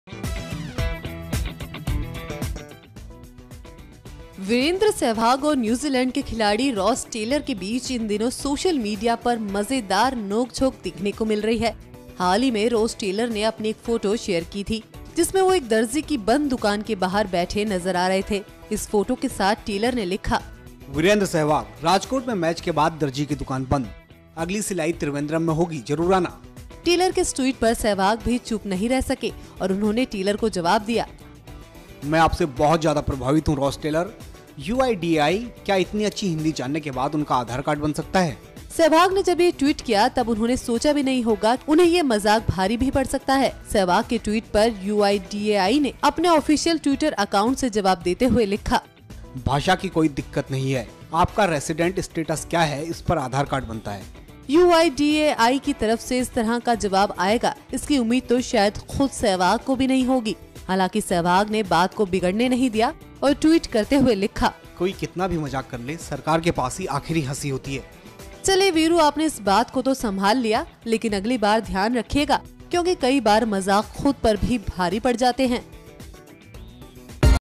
Aadhar Card वाला मजाक Virendr Sehwag को पड़ा भारी, Government ने दिया ऐसा जवाब
Ross Taylor has been involved in off-field banter with former India batsman Virender Sehwag, and Twitterati can't seem to be getting enough of it. During the ODI series Sehwag had referred to Kiwi batsman as 'Darji', and since then the duo have exchanged some hilarious tweets.